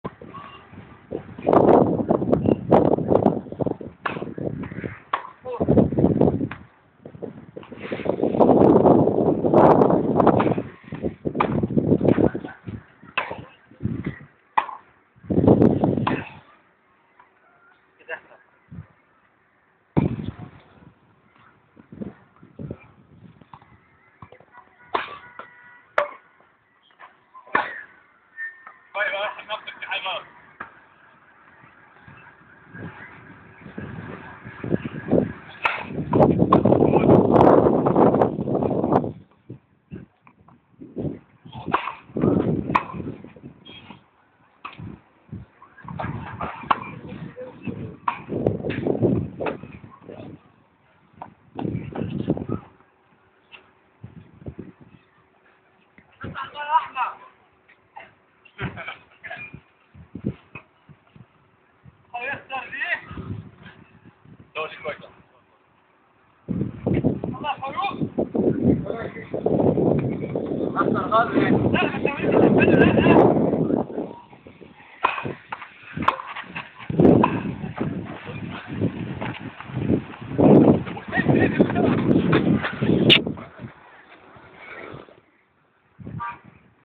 Bosnian tennis sensation Sherifov Hassanovic
Sherifov Hassanovic,U-18 #1 ranked player in Bosnia. ITF ranking # 63.Sherifov's father was a former Sovite Union Davis Cup Player.